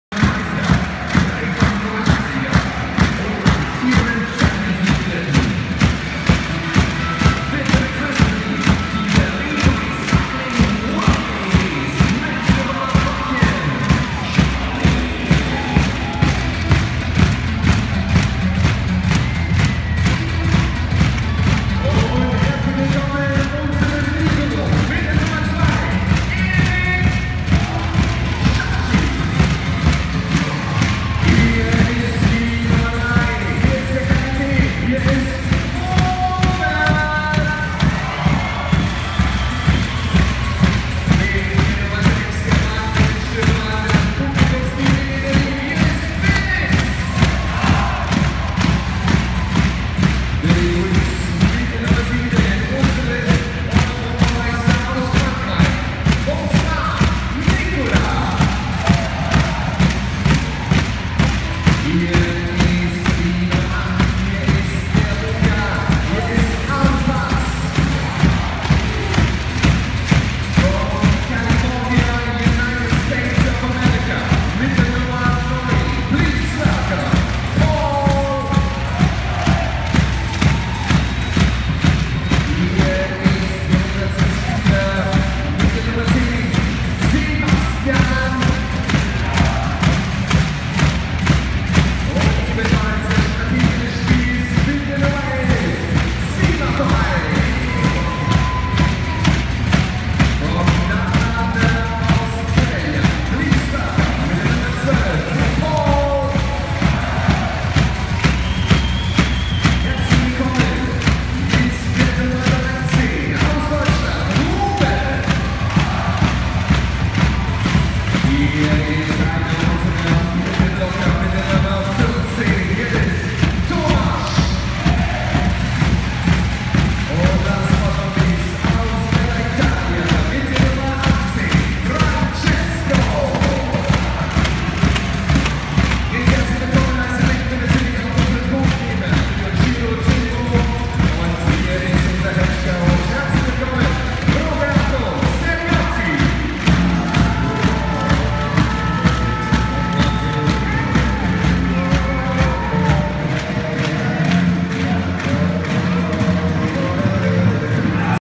Volleyball Bundesliga BR Volleys einkaufen der Mannschaft am 28.11.2015
Volleyball Bundesliga BR Volleys gegen TV Rottenburg am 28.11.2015 in der Max Schmeling Halle Berlin.
Einlaufen der Mannschaft. Endstand 3:0 für die Berliner